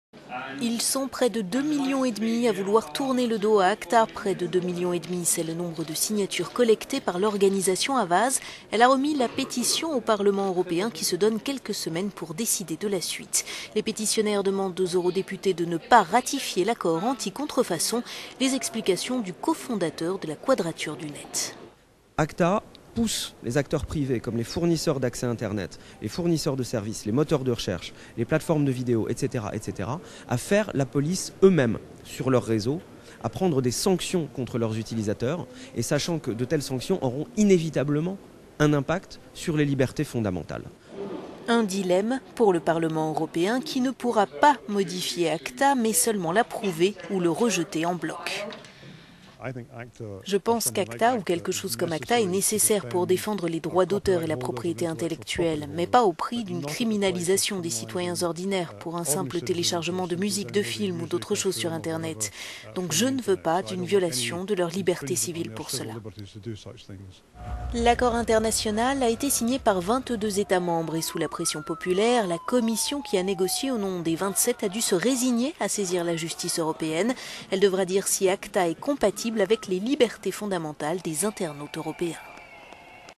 Une pétition anti-ACTA
Ils sont près de deux millions et demi à vouloir tourner le dos à ACTA. Près de deux millions et demi, c'est le nombre de signatures collectées par l'organisation Avaaz. Elle a remis la pétition au Parlement européen qui se donne quelques semaines pour décider de la suite. Les pétitionnaires demandent aux eurodéputés de ne pas ratifier l'accord anti-contrefaçon. Les explications de Jérémie Zimmermann, co-fondateur de la Quadrature du Net